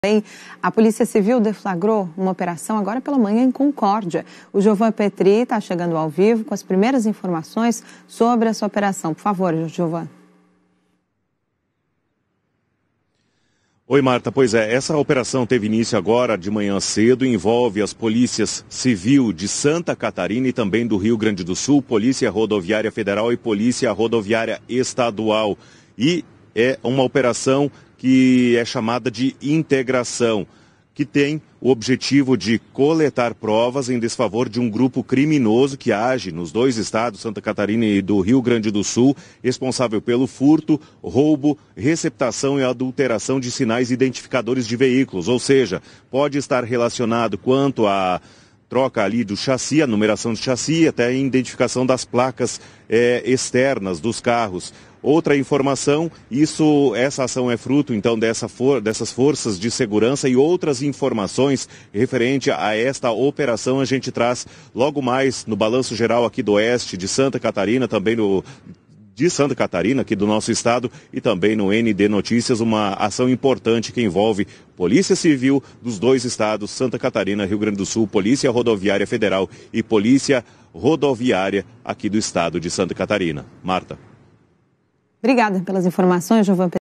Polícia Civil cumpre mandados na manhã desta terça-feira em Concórdia
Acompanhe os detalhes da 'operação Integração' deflagrada na manhã desta terça-feira (20) para combater furto, roubo, receptação e adulteração de sinais identificadores de veículos em SC e no RS.